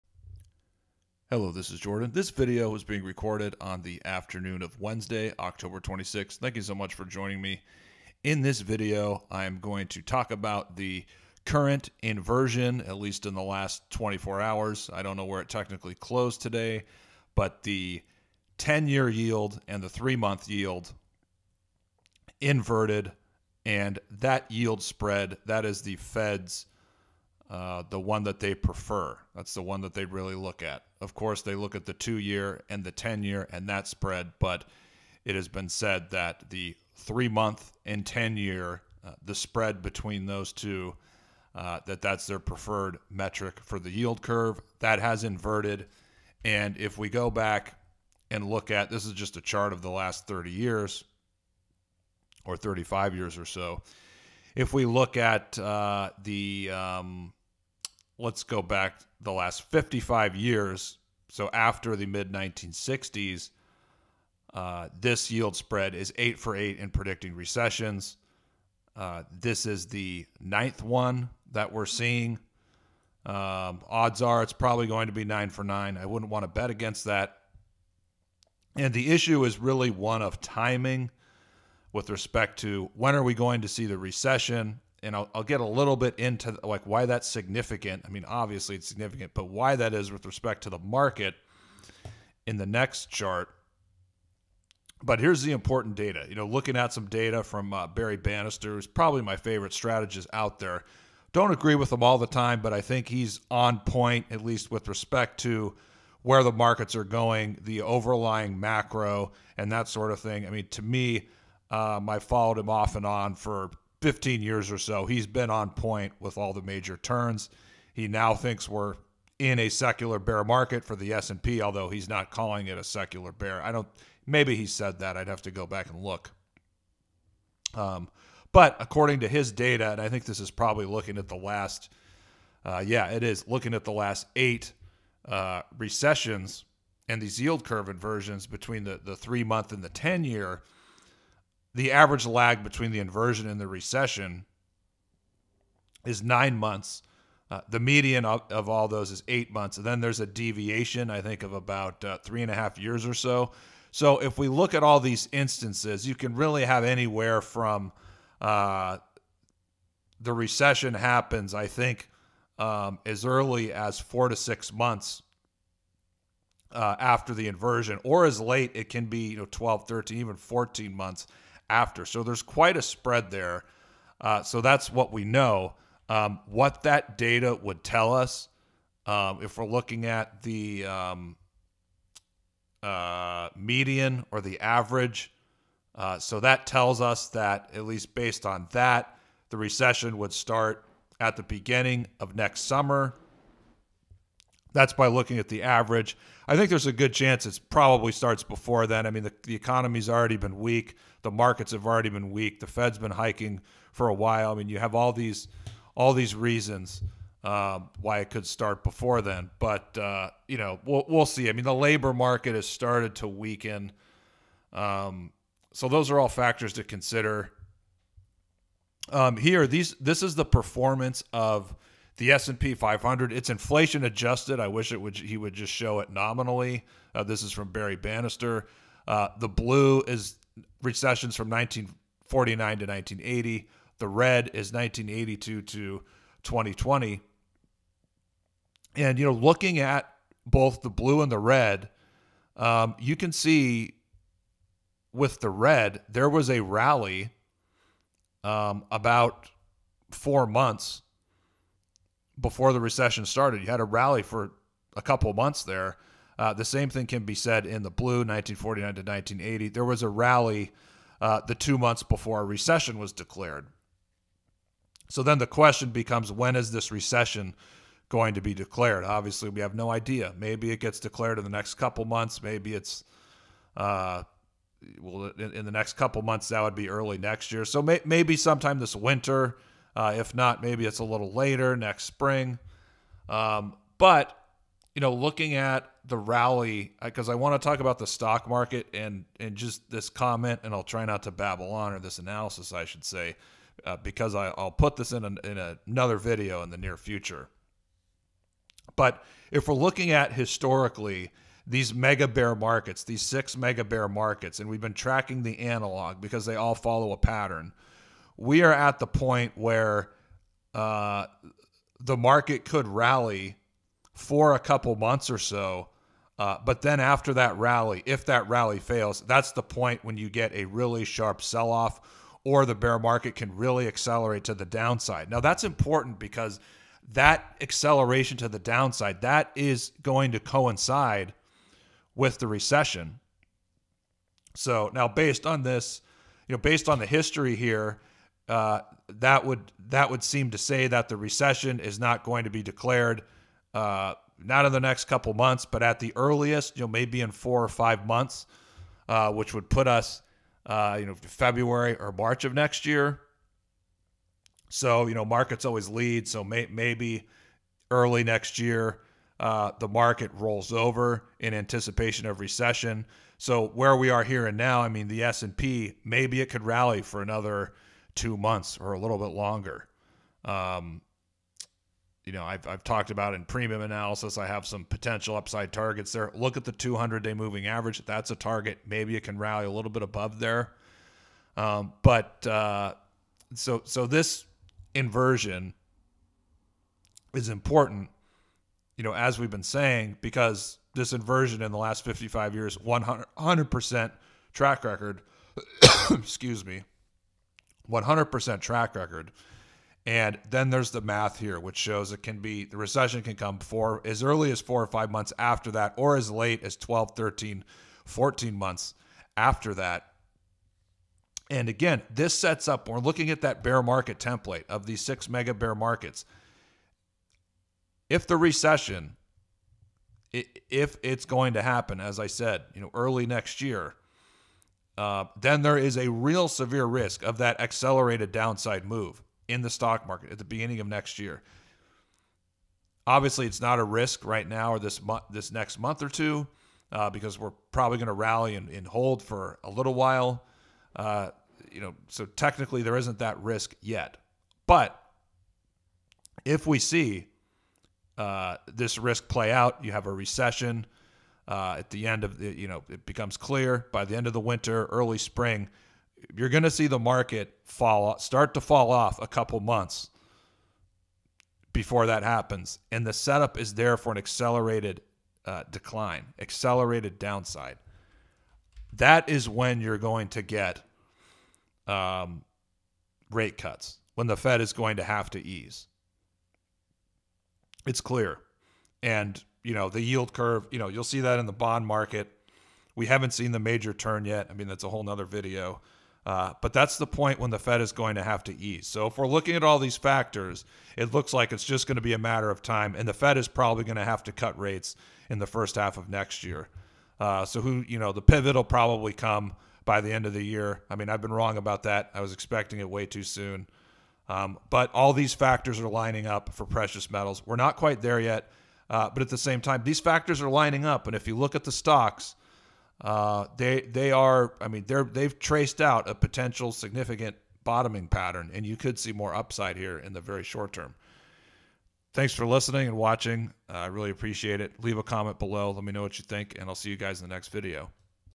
Federal Reserve Indicator of Recession Just Hit Today
The Federal Reserve's preferred yield spread, the 3-month to 10-year yield inverted today. In the last 55 years there have been 8 previous inversions and 8 recessions. This yield spread is an even better indicator of recession than the 2-year to 10-year spread.

According to Barry Bannister, head strategist at Stifel, the median and average time between inversion and recession is 8 and 9 months. There is a standard deviation of almost 4 months. There are examples of recessions that occurred 4 to 6 months after the inversion as well as several recessions 10 to 12 months after inversion.

The probability now favors a recession beginning likely at the end of winter or end of spring 2023. The data shows that there is a big decline in the stock market at the start of the recession. This aligns with our take on the pattern of the 6 mega-bear markets. Should this rally in the S&P 500 last into year end but a recession gets declared later, it sets up a nasty start to 2023. If this plays out, look for rate cuts by the end of Q1 2023 or in Q2 2023.